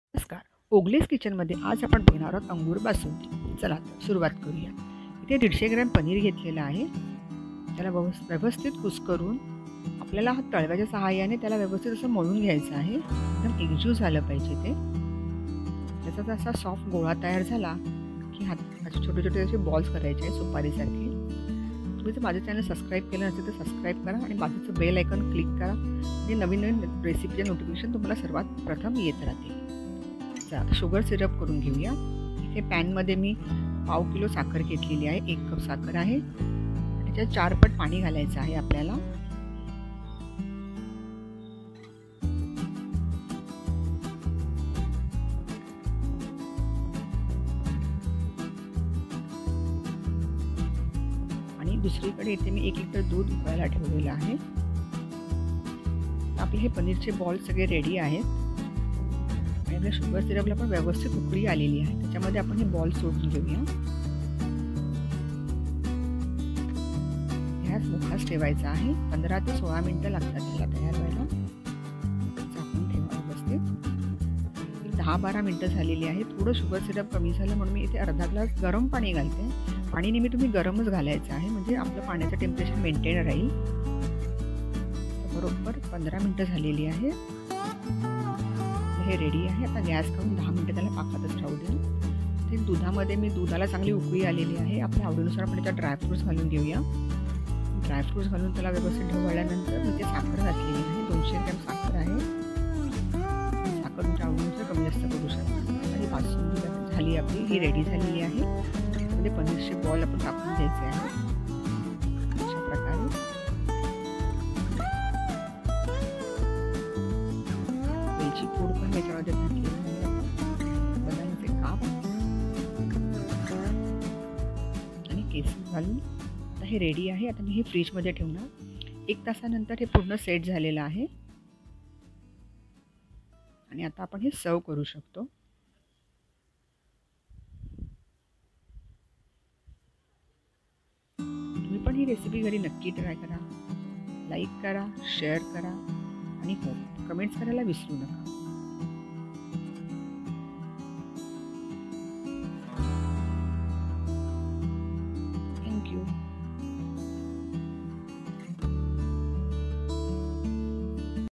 Angoori Basundi recipe | Tempting paneer sweet receipe | अंगुरी बासुंदी
Amazing sweet recipe to enhance your celebration;
Angoori Basundi 😋

Ingredients:

150 gm paneer
1/4 gm sugar
1 liter milk
dry fruits
200 gm sugar( for basundi)

For Sugar syrup:
1 cup sugar
4 cups water

Do try this recipe and share your comments!